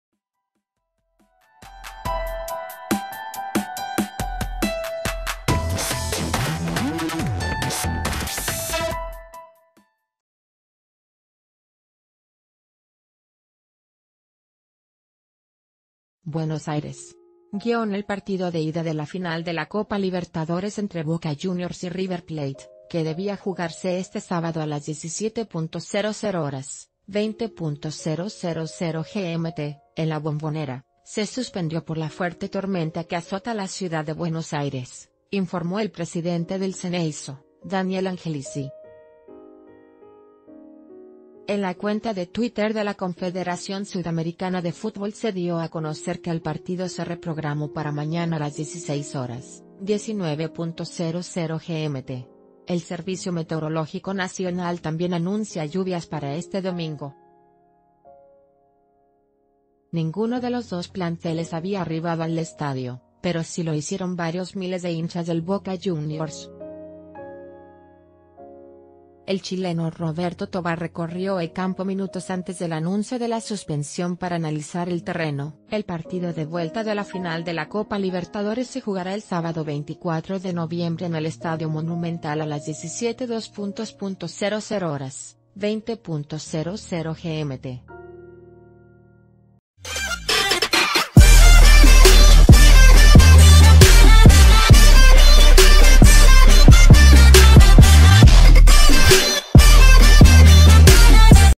Por intensas lluvias, suspenden partido de ida de la final Boca-River de la Libertadores
Buenos Aires.- El partido de ida de la final de la Copa Libertadores entre Boca Juniors y River Plate, que debía jugarse este sábado a las 17.00 horas (20.000 GMT) en la Bombonera, se suspendió por la fuerte tormenta que azota a la ciudad de Buenos Aires, informó el presidente del Xeneizo, Daniel An...